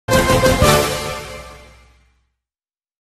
ID Rede SMT [Sistema Metropolitana de Televisão] (2022)
Vinheta da Rede SMT, Sistema Metropolitana de Televisão, a Ex-ABC e Ex-Sistema Moura de Teleivsão, q foi reativada em Janeiro desse ano, tendo uma nova fase na emissora.

Créditos:
JB+ Trilhas da TV ___________________________________________________________________________